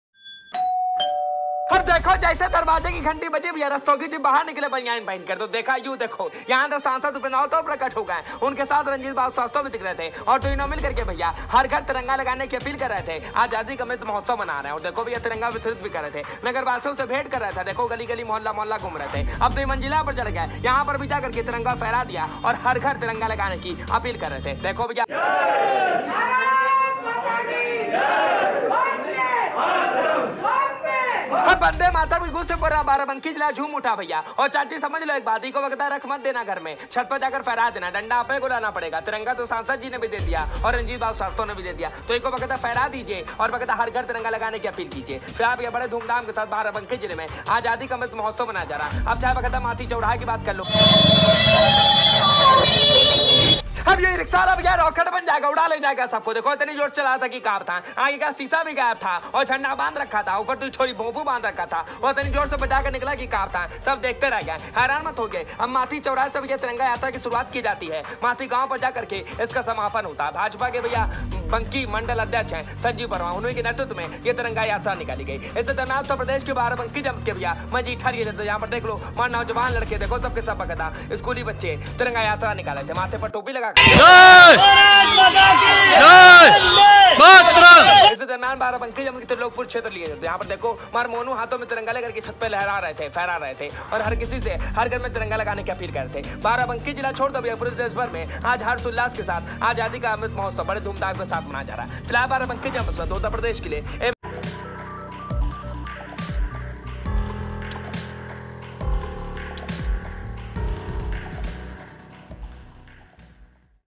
Barabanki में बजी घंटी प्रकट हुए सांसद व चेयरमैन घर-घर तिरंगा !
बाराबंकी में 15 अगस्त को लेकर के तैयारियां तेज हर घर तिरंगा लगाने की नेता मंत्री कर रहे
नगर के गली मोहल्लों में पहुंचे सांसद व चेयरमैन हर घर तिरंगा लगाने की कर रहे हैं अभी वितरित कर रहे